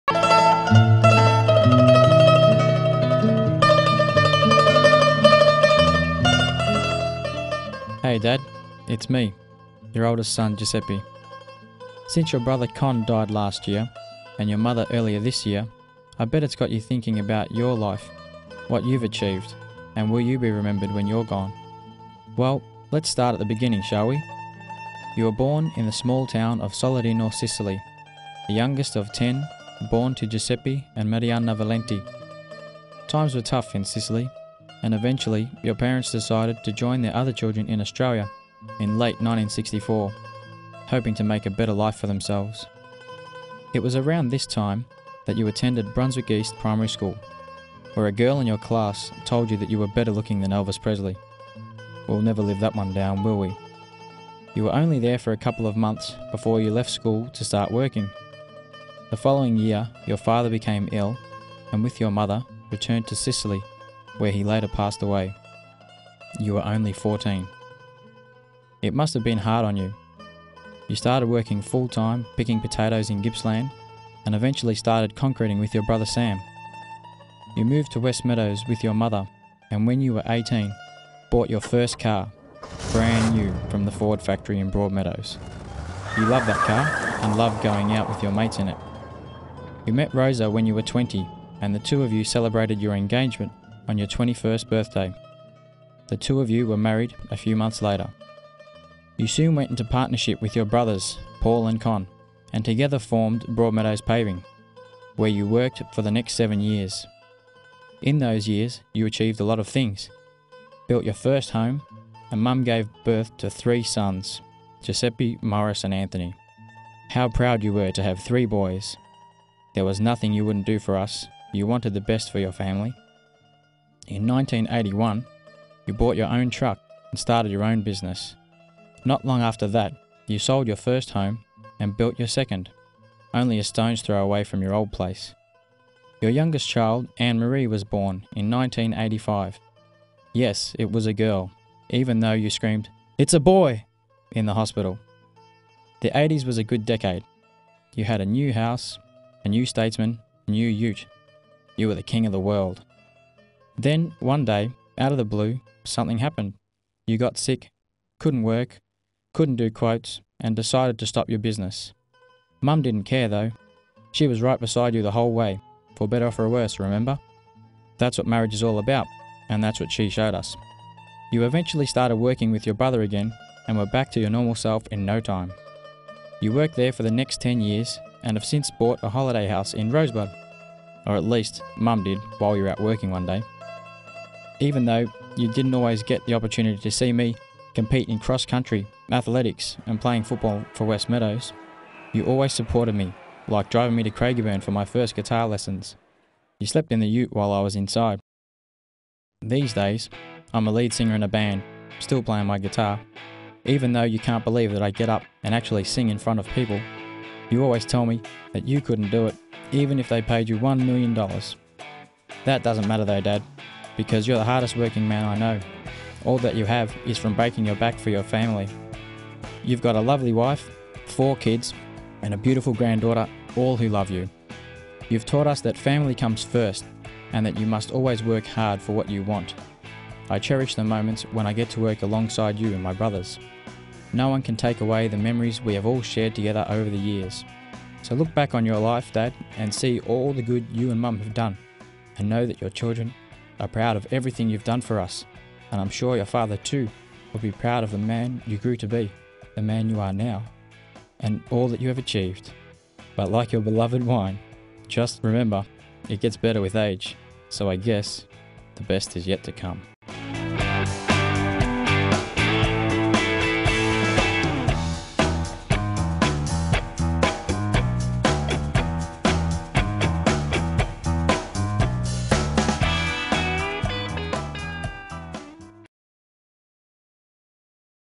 The Goodfather - Sebastiano Valenti
Joe's dad, Sebastiano Valenti migrated to Australia when he was a young boy, Joe wanted to celebrate his father's life achievements and created a moving tribute to him.  "I am so proud of my dad's achievements.  I wanted to let him know how he has affected my life and who I am as a person".  His favourite movie is The Godfather, but to me he will always bee The Goodfather...  Here's a short story I did on dad back in 2005.

Sebastiano Valenti (b.1952) migrated from the town of Solarino, Sicily to Melbourne, Australia in 1964. His father died when he was only 14 years old, from there he stopped school and started working in Gippsland picking potatoes.  A few years later he would start concreting, go into partnership with his two brothers Paolo and Concetto.  Married his wife Rosa in 1973, they had 4 children: Giuseppe (1975), Maurice (1977), Anthony (1979), and Anne-Marie (1985).  He eventually starting his own business Sebastian Valenti Paving.  2012 saw him celebrate his 60th birthday.  All his children are now married, and he's currently the proud grandfather to four granddaughters.  Life is good...